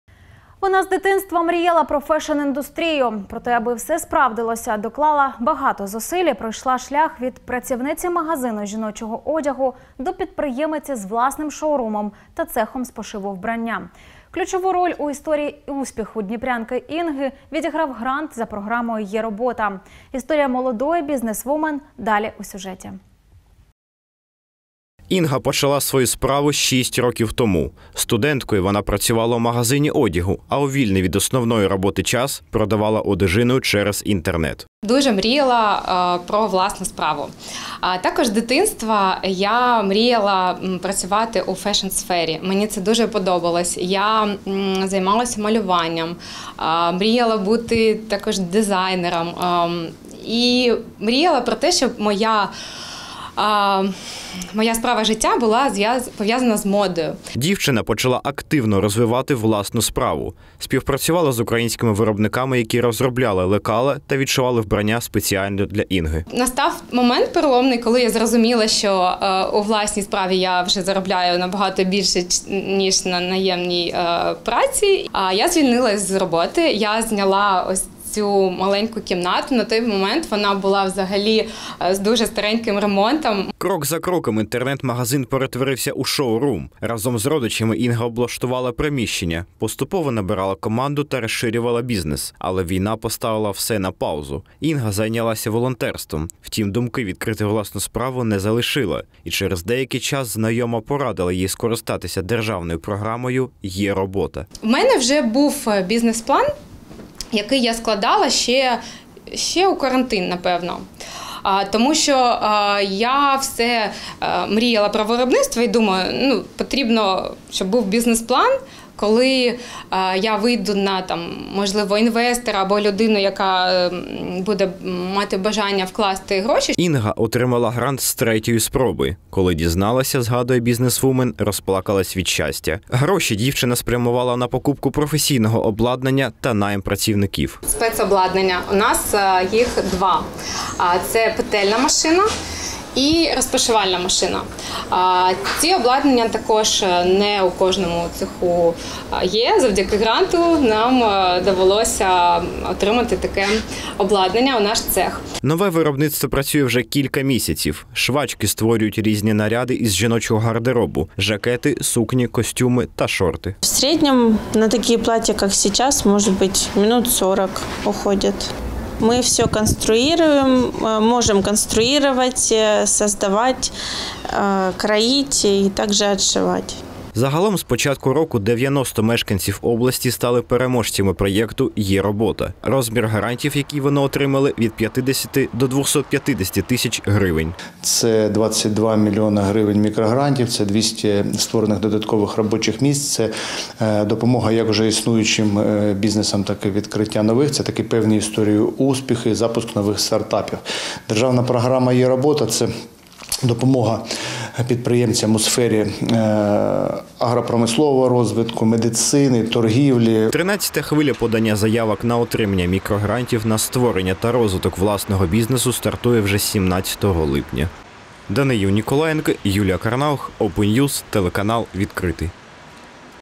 Дніпрянка виграла грант та запустила швацьке виробництво
Вона з дитинства мріяла про фешн-індустрію. Проте аби все справдилося, доклала багато зусиль і пройшла шлях  від працівниці магазину жіночого одягу до підприємиці з власним шоурумом та цехом з пошиву вбрання.  Ключову роль у історії успіху дніпрянки Інги відіграв грант за програмою «єРобота». Історія молодої бізнесвумен - далі у сюжеті.